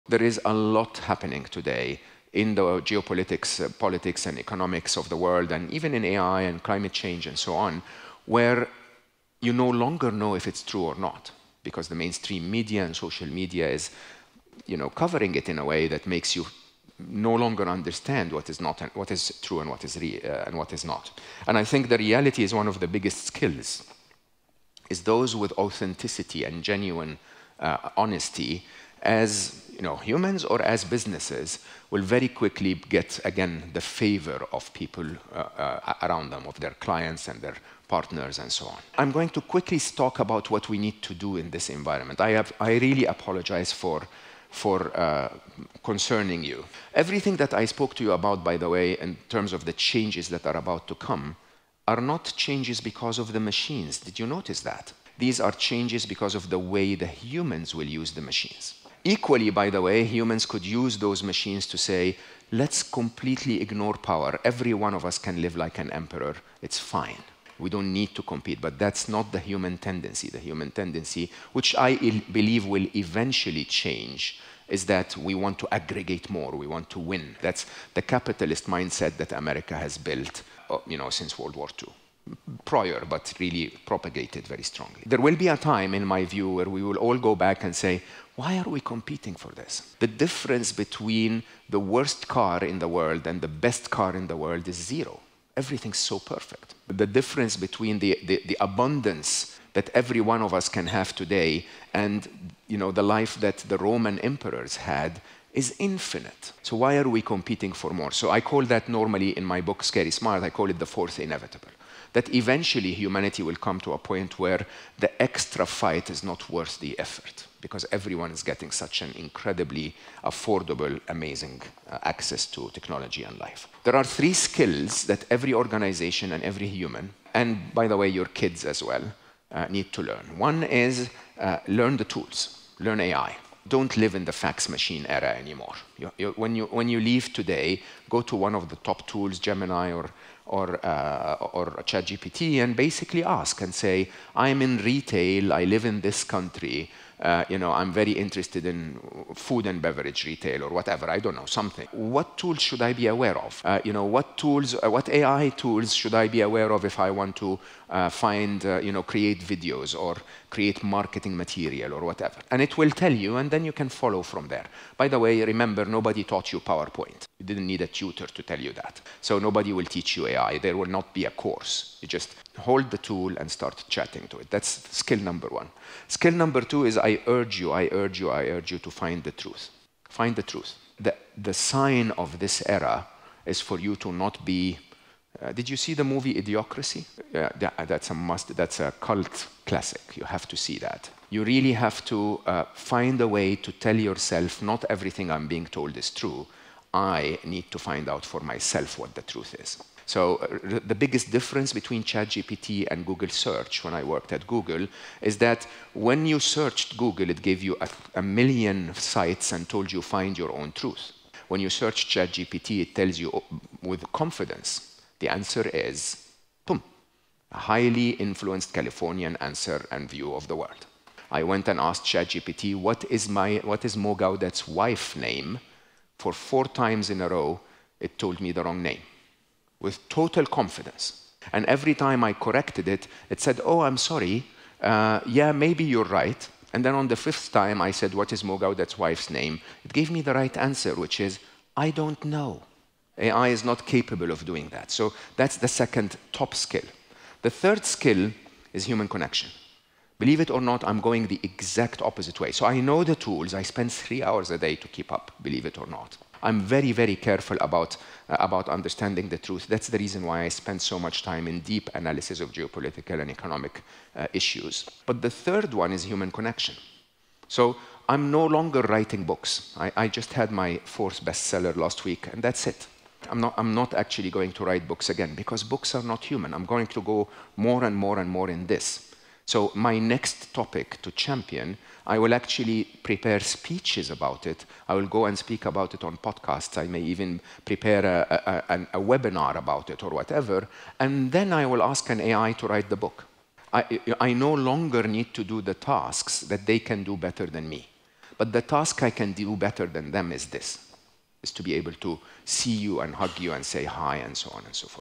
Mo Gawdat on AI: How Will AI Change Our Daily Lives?
The rapid evolution of technology, particularly artificial intelligence, is reshaping societal structures, human connections, and our understanding of truth, highlighting the urgent need for individuals to master digital tools, seek authenticity, and prioritize genuine human relationships in an increasingly automated and complex world.

Three essential skills for the future: master the tool, seek the truth, cultivate human connection